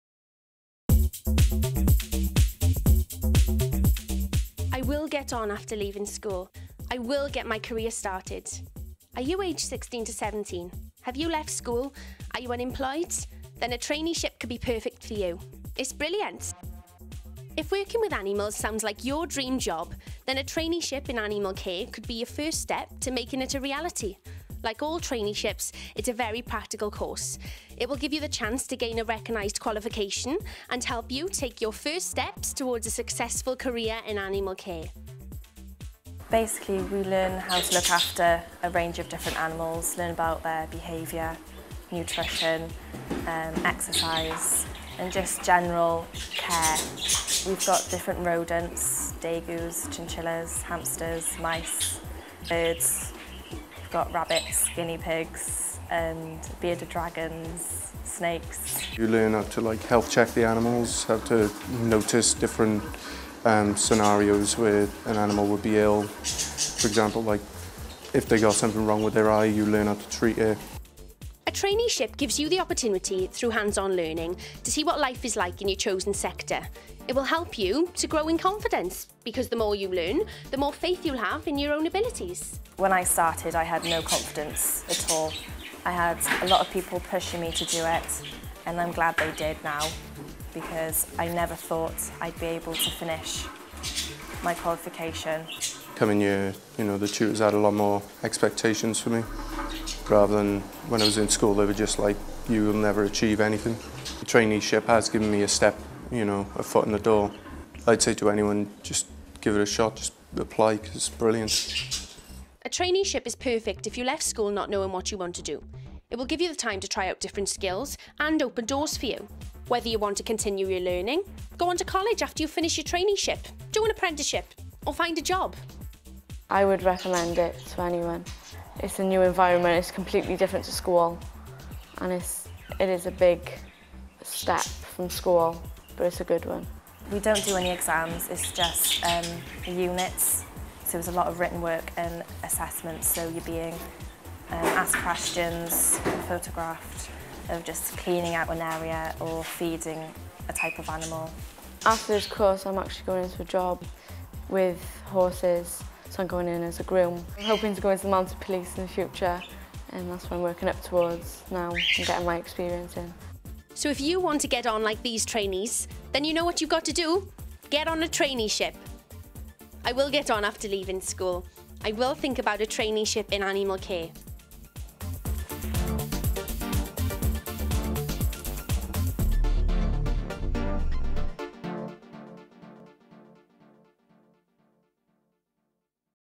Traineeships - Animal Care
Love the thought of working with animals? A Traineeship could be your first step towards getting a great job working in Animal Welfare. Check out this video to see how! If Animal Care is not for you, then don't worry. Just have a look at our other videos covering Traineeships in Construction, Health & Social Care, Catering, Child Care, Mechanics, Hairdressing and IT and see what's best for you.